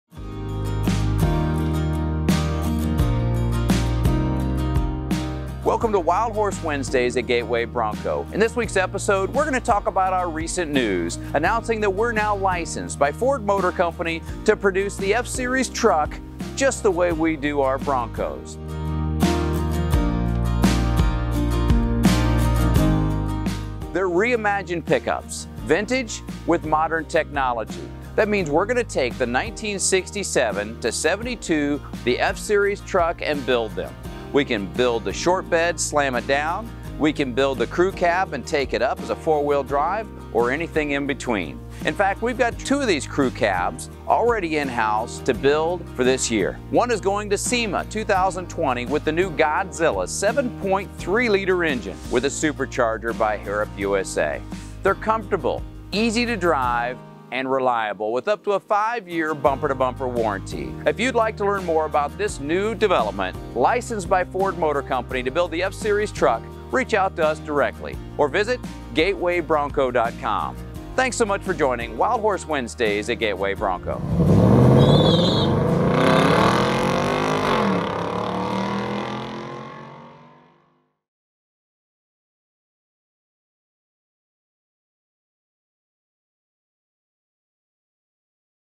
Wild Horse Wednesdays - Episode 13 - F-Series Announcement - Now building custom F-Series Trucks
In this episode we have a MAJOR ANNOUNCEMENT! Watch to find out what it is! (Hint: Ford license and F-Series)